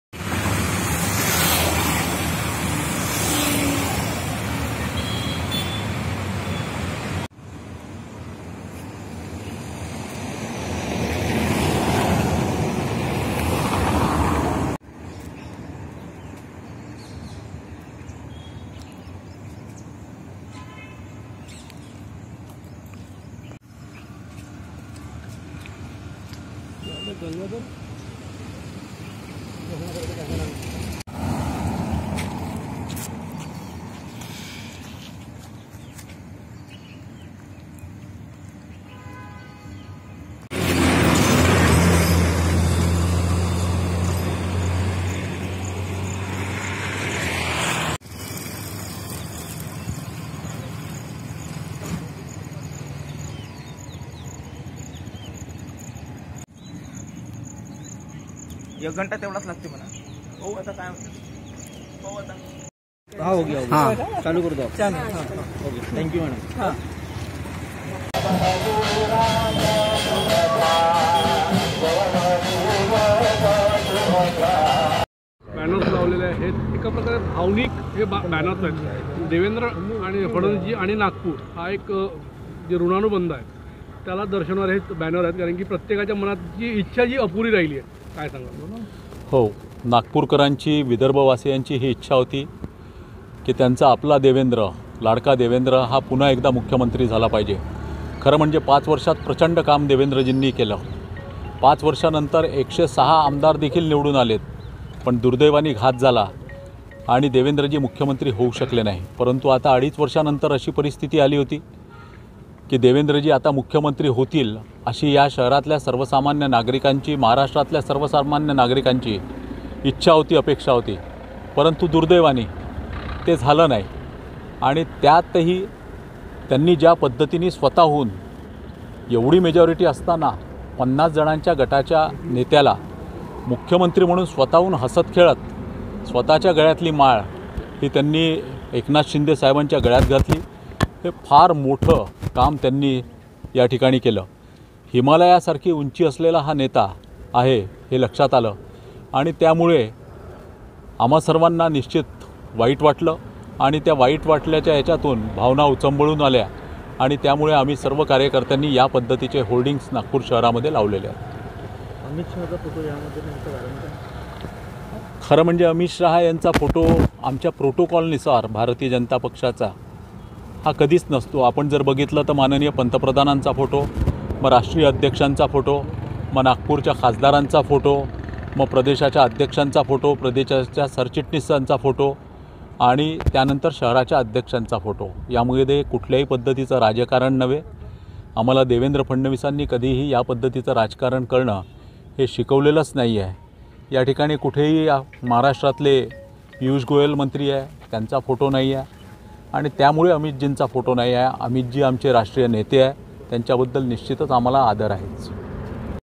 NAGPUR | नागपुरात देवेंद्र फडणवीस यांना शुभेछ्या देणारे लावल्या गेले भावनिक होर्डिंग
उपमुख्यमंत्री देवेंद्र फडणवीस मुख्यमंत्री व्हावे ही आमची अपेक्षा होती.परंतु देवेंद्र फडणवीस यांनी मुख्यमंत्री पद स्वतः एकनाथ शिंदे यांना दिला. देवेंद्र फडणवीस साठी लागलेल्या होर्डिंग्ज मध्ये अमित शहा यांच्या फोटो दिसत नाही त्याची चर्चा जास्त होत आहे.या होर्डिंग्ज बदल नागपूरचे माजी महापौर संदीप जोशी यांचा यांनी सांगितले की हा होर्डिंग हे भारतीय जनता पक्षाचे प्रोटोकॉल च्या हिशोभाने लावले आहे.

बाईट - माजी महापौर संदीप जोशी यांनी लावले होर्डिंग